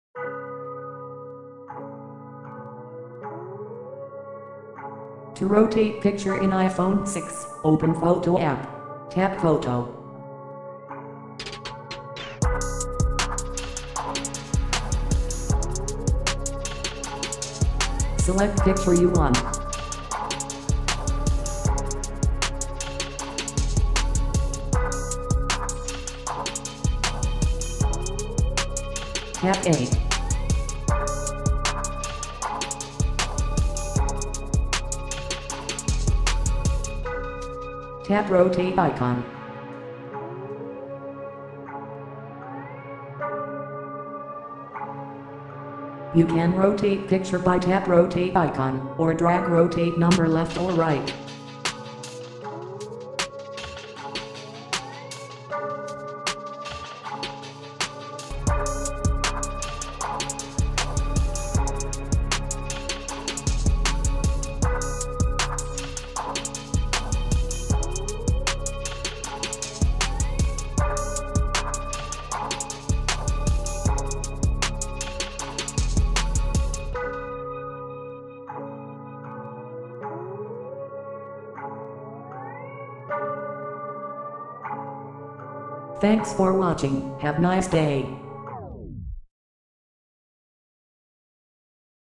How to rotate picture in iPhone 6
On this video you can know How to rotate picture in iPhone 6 with iOS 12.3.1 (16D39) operating system